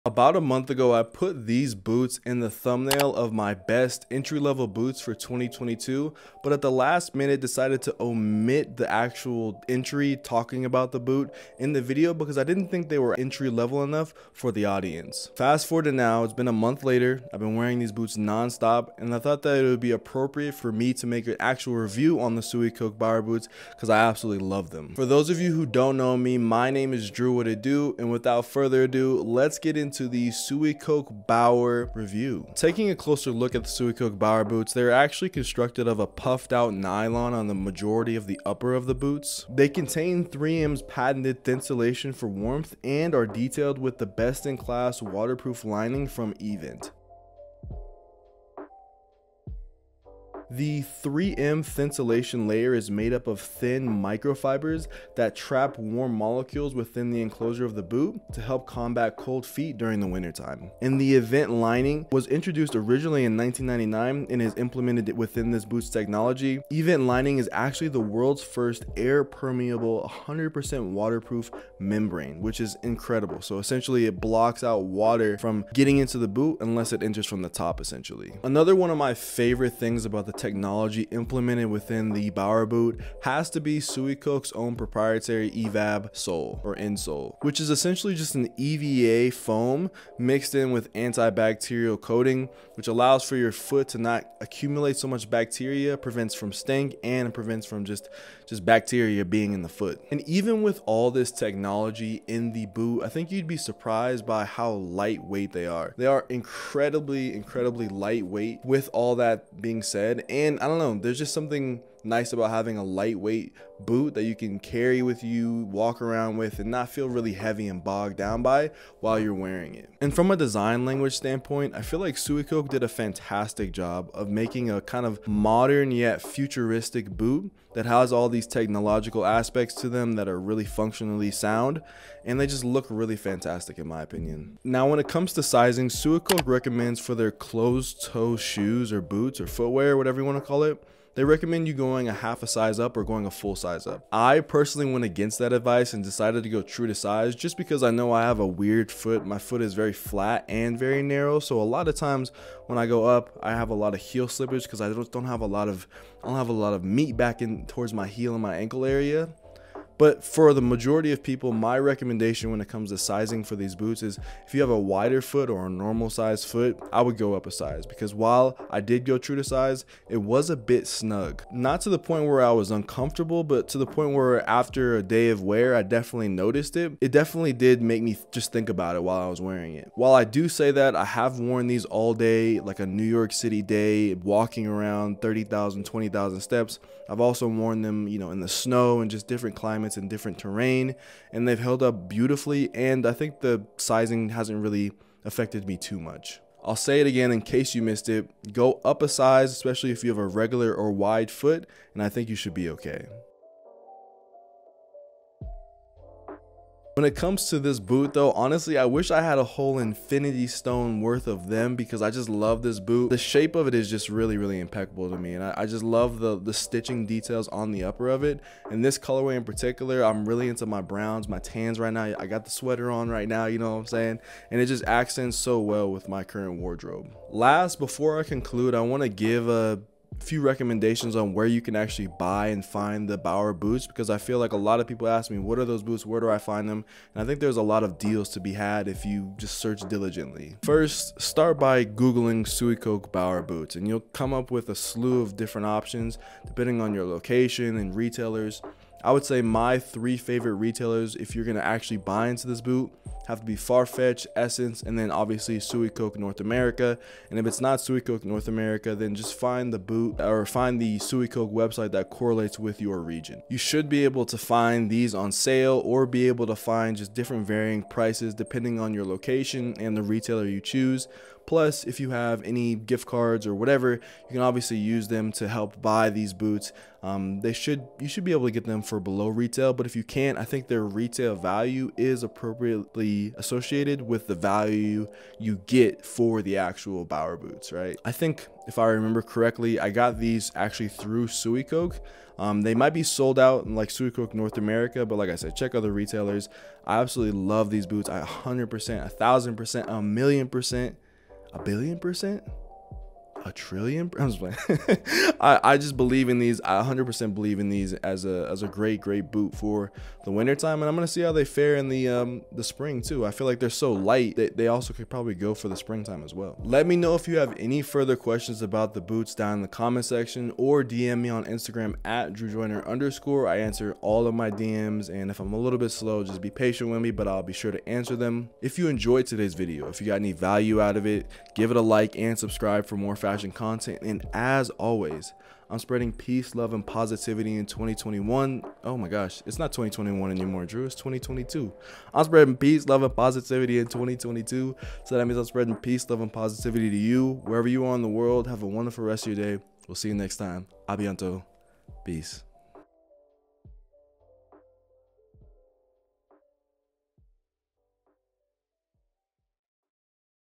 Suicoke Bower Boot (Sizing + on Feet)
The Suicoke Bower Boots are some of my favorite boots out now! In this video, I review where you can buy them, sizing, and the overall features of the boots.

Pants in Video

Chapters:

0:00 Intro
0:30 Materials/Features
2:15 Sizing/On Feet
3:45 Where to Buy

Measurements:

Height: 6' 3"
Waist: 33"
Waist to Ankle (Outseam): 34"
Inseam: 32.5"
Shoe Size: US 11

Camera Gear/Set Up:

Editing Laptop
Canon M50
Lavalier Microphone
Condensor Microphone
Shotgun Microphone
Iphone Phone Case